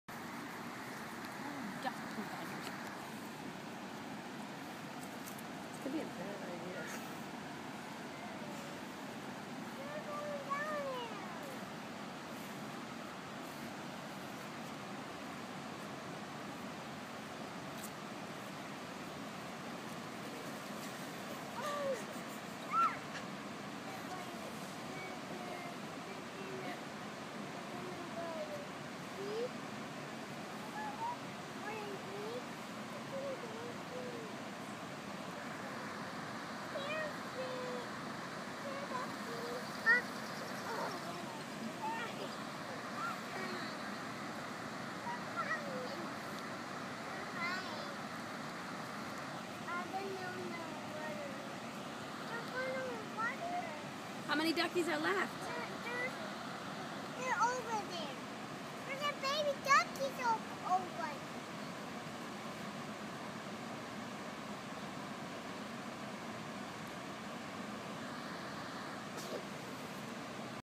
Ali, Morgan, and the Ducks
Ali and Morgan attempt to make friends with the ducks on the banks of the Red Cedar at Michigan State University.